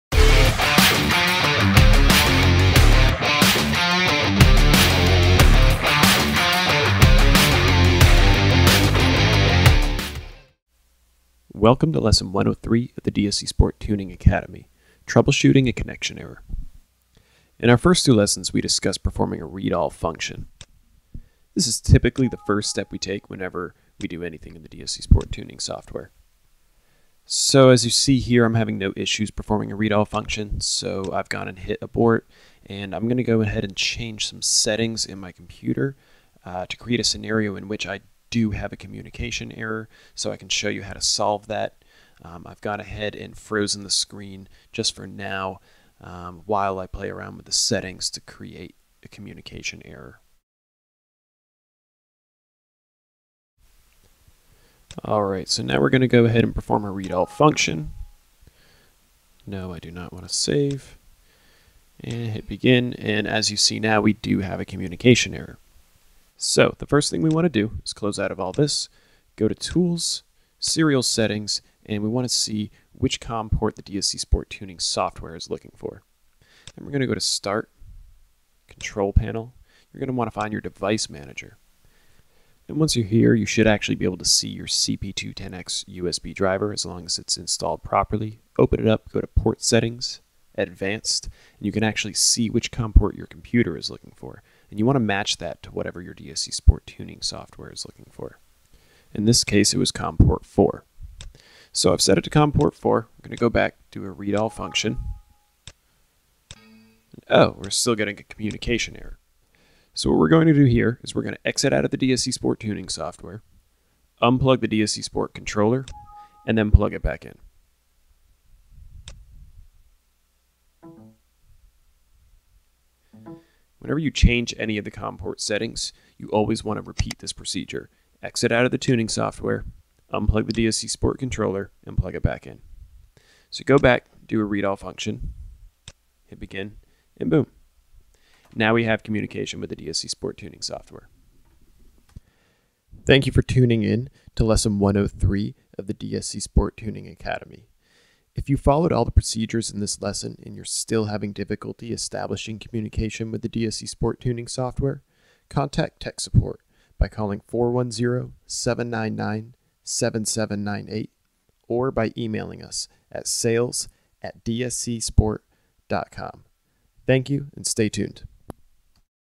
DSC Sport Tuning Academy | Lesson 103 - "Troubleshooting a Connection Error"
We're back with another episode of the DSC Sport Tuning Academy! In Lesson 103, we walk you through how to troubleshoot a connection error.

Particularly on initial installation, some users experience difficulty establishing communication with the DSC Sport Tuning Software. This is usually due to a mismatch between the COM Port your computer is using for the CP210x USB driver and the COM Port the DSC Sport Tuning Software is looking for. In this lesson, we walk you through how to match your computer's COM Port settings to that of the Tuning Software. Reminder: Always close out of the Tuning Software and disconnect and reconnect the DSC Sport controller after making any COM Port changes!